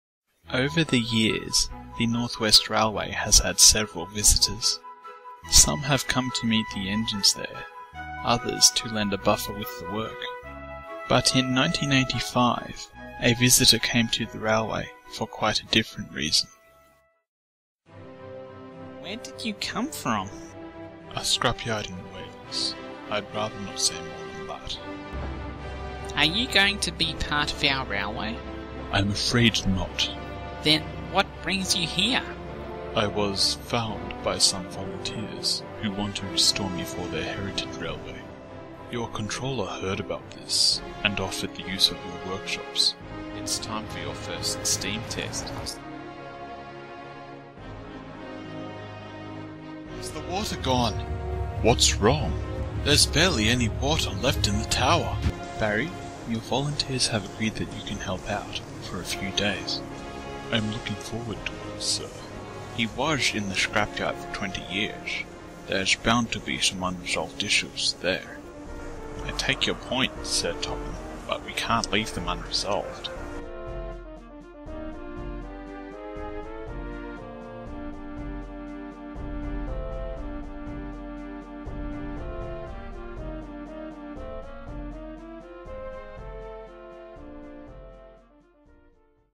Sodor the Early Years: Barry the Rescue Engine - Trailer
Exactly what it says on the box, the trailer for my upcoming four-part miniseries.

Models & Routes by:
Sodor Island 3D
The Sudrian Community
Northwestern Railways
Paul Hobbs
Skipper1945

Thomas and Friends copyright Hit Entertainment. No profit is being made from this video.